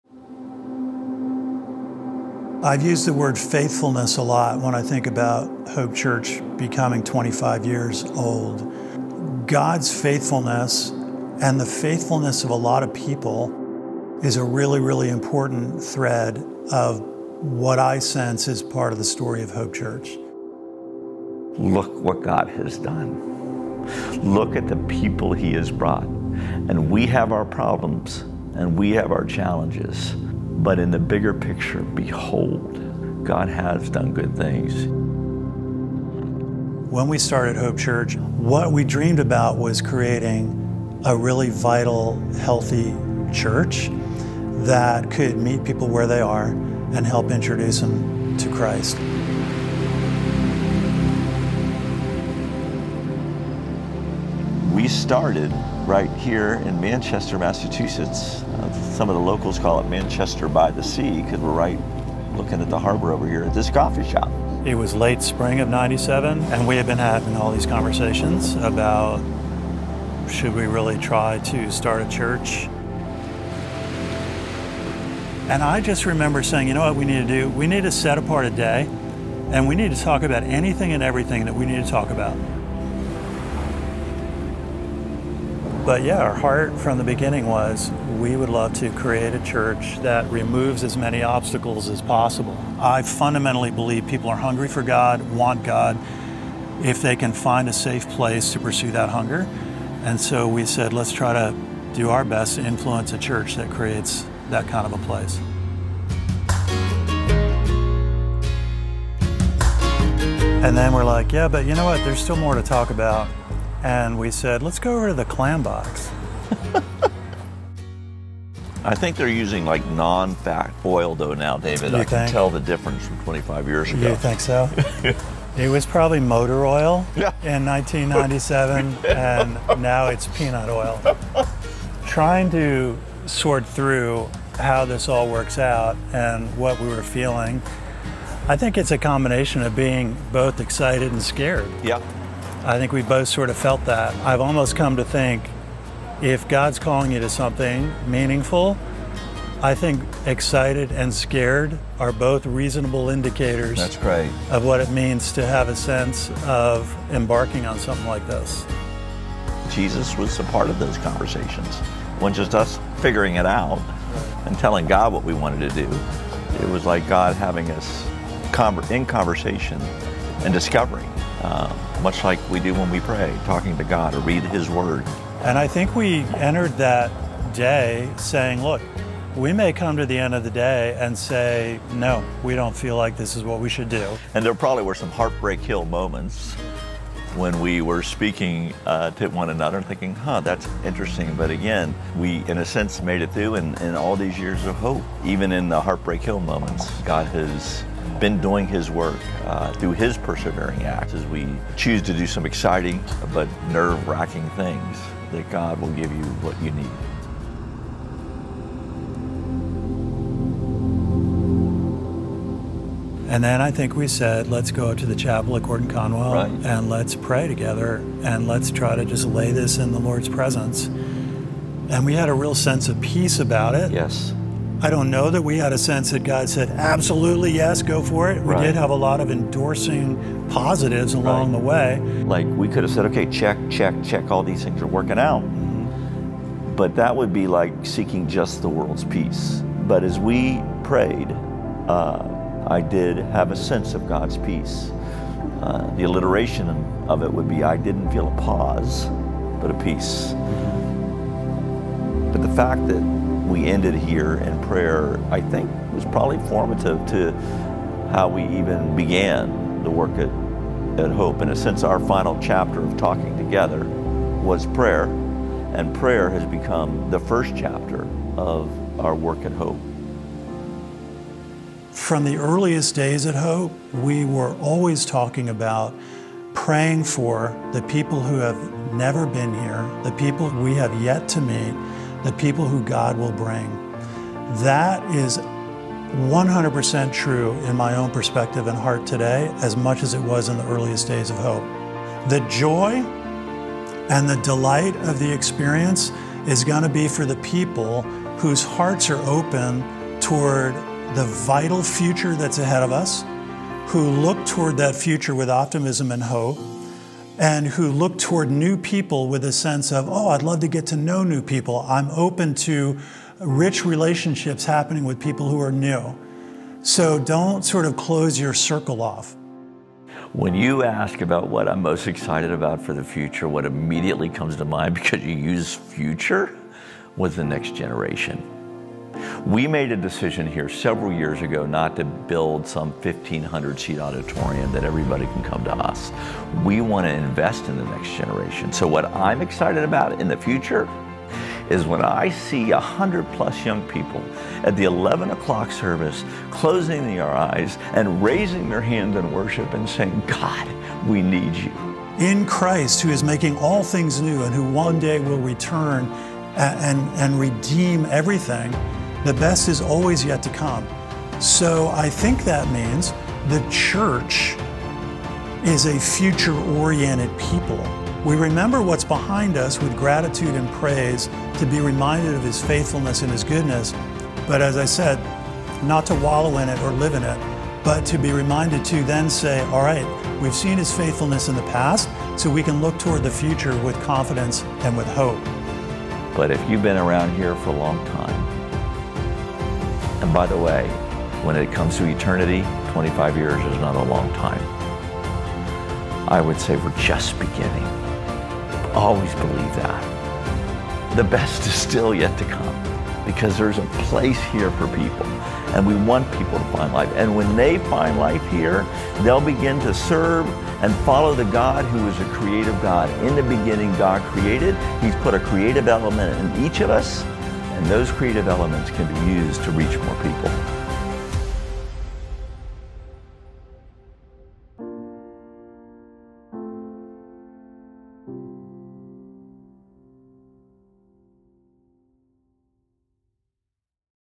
HOPE’S 25TH Anniversary
As we celebrate 25 years of HOPE, hear from pastors David Dwight and Pete Bowell as they reflect on God’s faithfulness and look forward to the best that is yet to come.